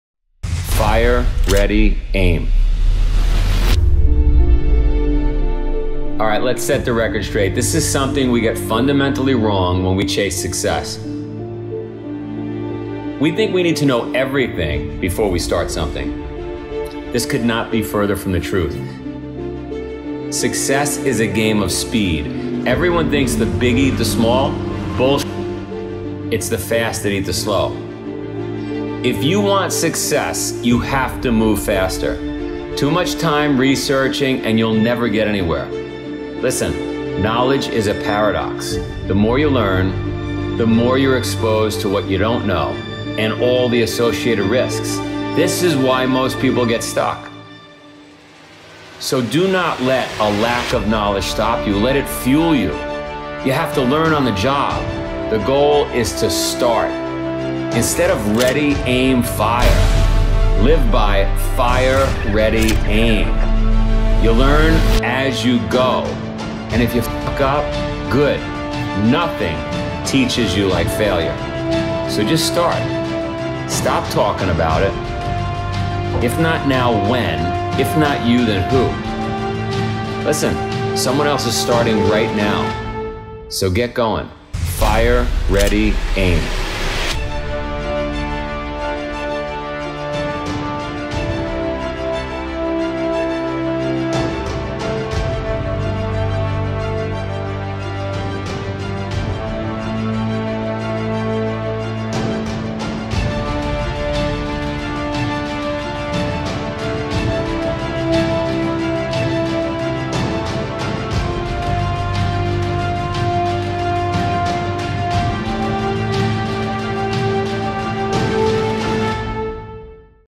START NOW - Powerful Motivational Speech | Spartan
START NOW! Fire. Ready. Set. Don't wait. Don't delay. Just go. If not now then when? Powerful New Motivational Speech by Spartan Race Founder & CEO Joe De Sena.

►Speaker

Joe De Sena